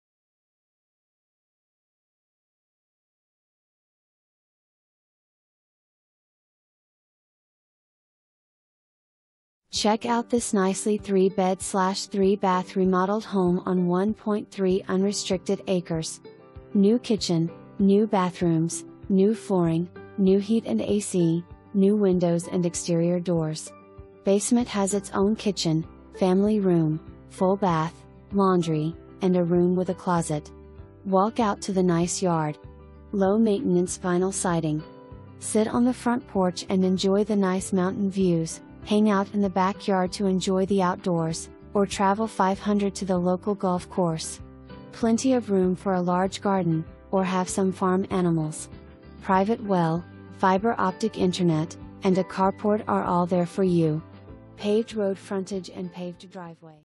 610 COLD BRANCH ROAD
Check out this nicely 3bd/3ba remodeled home on 1.3 unrestricted acres! New kitchen, new bathrooms, new flooring, new heat and A/C, new windows & exterior doors. Basement has it's own kitchen, family room, full bath, laundry, and a room with a closet. Walk out to the nice yard. Low maintenance vinyl siding. Sit on the front porch and enjoy the nice mountain views, hang out in the back yard to enjoy the outdoors, or travel 500' to the local golf course! Plenty of room for a large garden, or have some farm animals! Private well, Fiber Optic internet, and a carport are all there for you! Paved road frontage and paved driveway.

3 full bath

Carol Robinson
Coldwell Banker High Country Realty